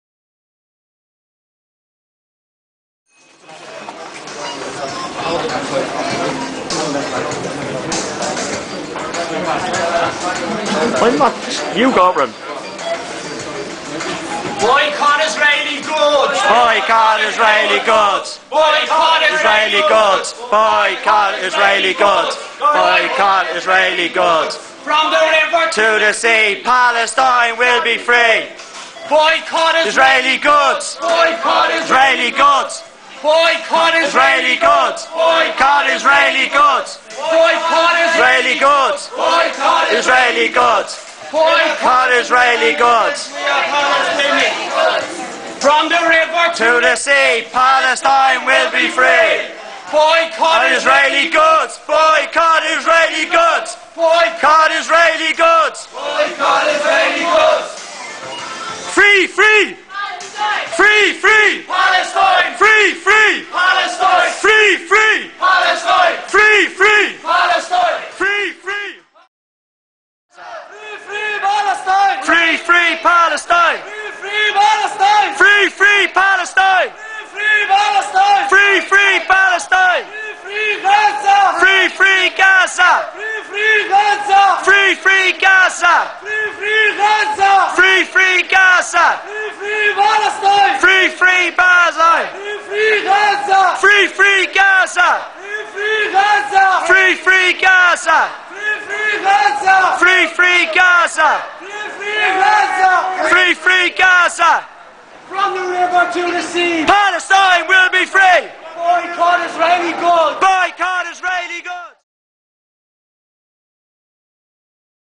Éirígí Activists Take Part in a Boycott Israeli Goods Protest in M&S on Grafton Street in Dublin
Éirígí activists take part in a Boycott Israeli Goods protest in Marks & Spencers on Grafton Street in Dublin.